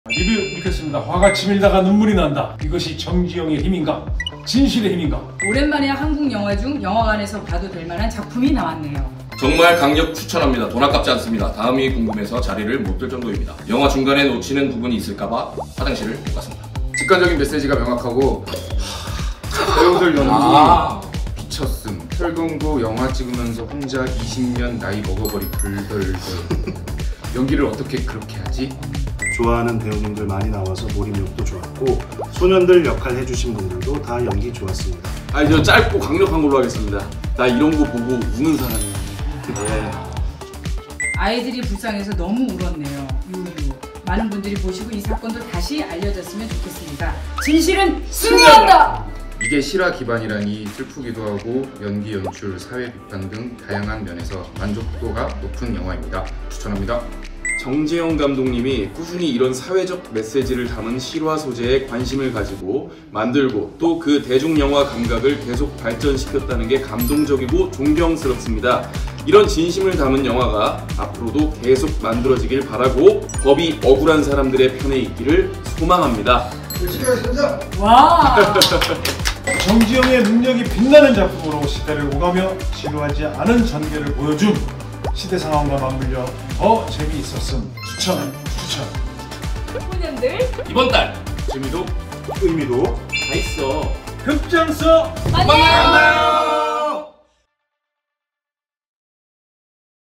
진실은 언제나 승리한다!! CGV 골든에그 95% [소년들] 에그소년단이 직접 읽어주는 실시간 리뷰!
뽀짝한 소년들 단체티부터 정지영 감독님의 깨알 '열심히 하겠습니다!' 다짐까지👊